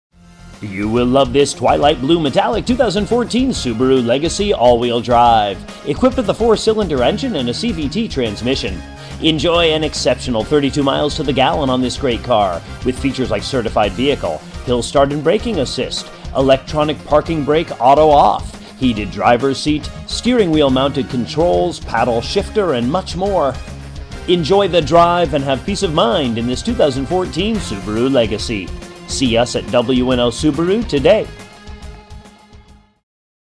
2014 Subaru Legacy W & L Subaru Northumberland, PA 17857
Subaru Legacy Imagine driving this  Twilight Blue Metallic   Subaru Legacy Sedan AWD, equipped with a 4 Cylinder engine and a cvt transmission.
MILES: 24,750
VIN:4S3BMBC67E3021684